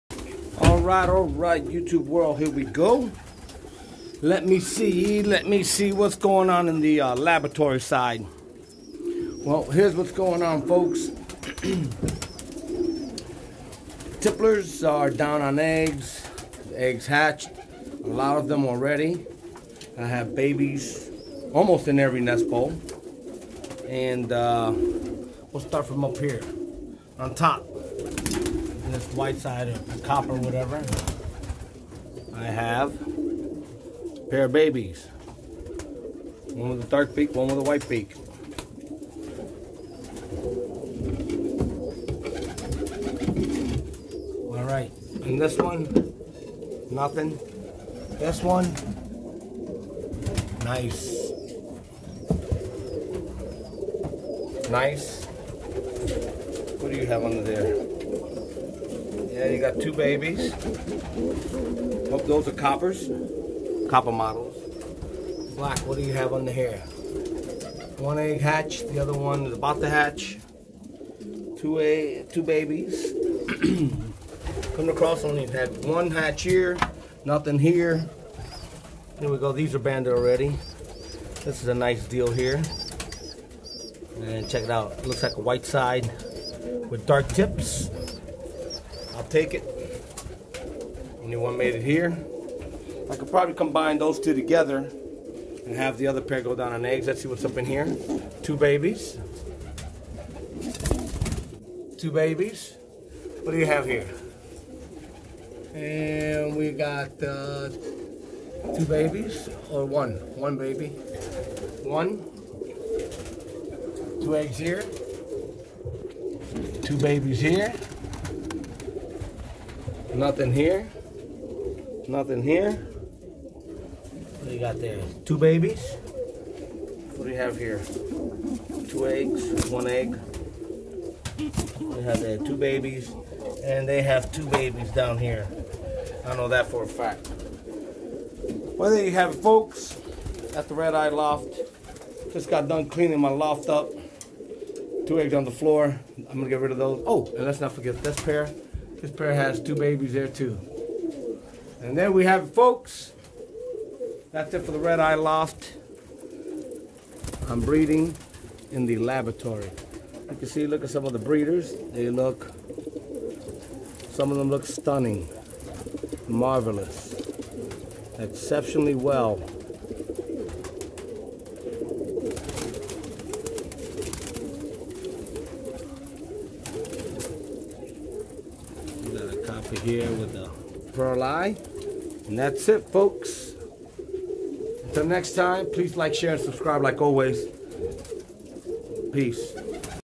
An update on the laboratory, Tipplers are growing.
Red Eye Loft breeding in the laboratory.